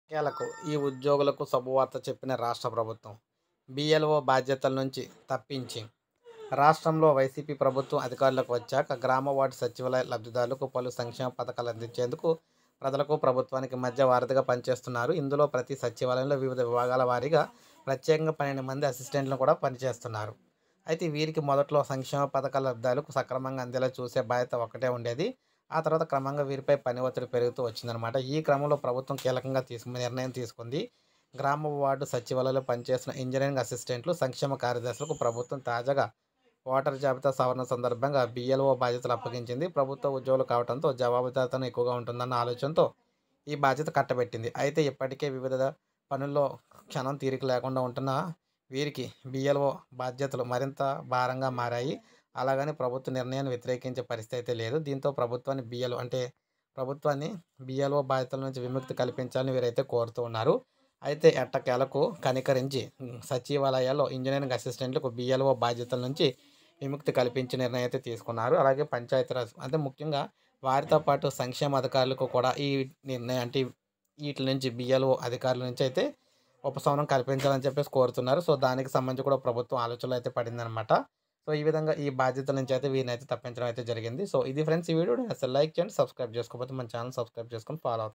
ఈ ఉద్యోగులకు అదనపు బాధ్యతల నుండి విముక్తి ap government employees latest update today.
hello everyone today update is AP government latest update on employees latest update .@Anandsocialzone